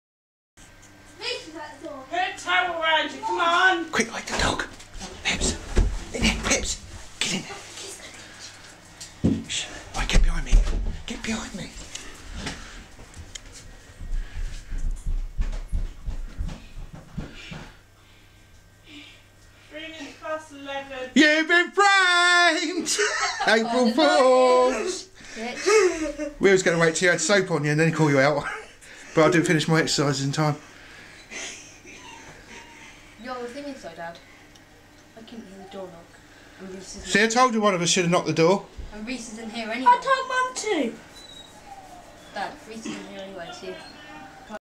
Andrew's Been Framed
April fooled Andrew when we told him his friend was at the door while Andrew was in middle of having a shower